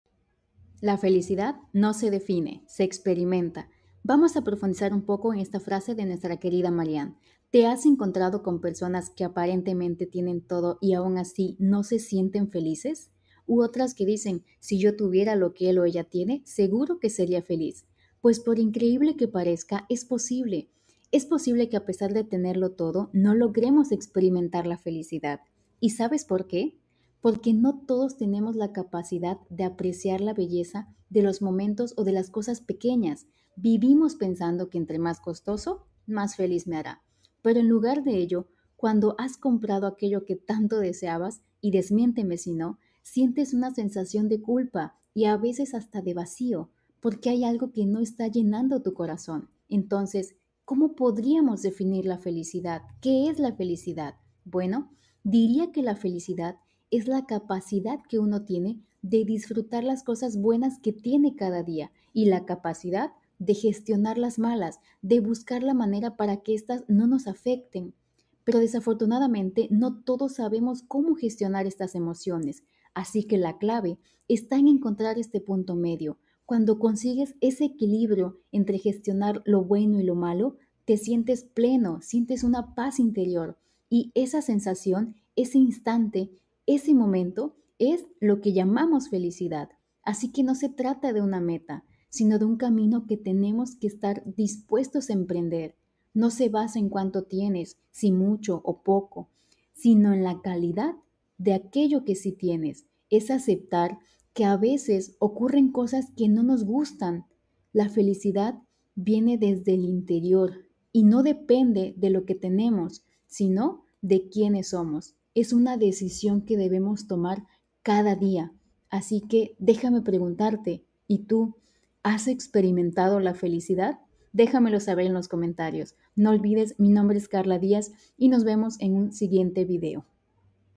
😃😁LA FELICIDAD NO SE DEFINE SE EXPERIMENTA | MARIAN ROJAS 😊😄
Hoy hablaremos sobre la felicidad, pero cómo podríamos definirla, bueno , como la capacidad para gestionar lo bueno y lo malo que nos pasa en la vida, la felicidad esta en las pequeñas cosas.
estamos constantemente en busca de la felicidad, y olvidamos que tenemos las cosas para ser feliz. que estan a nuestro alcance.

tu como definirias la felicidad.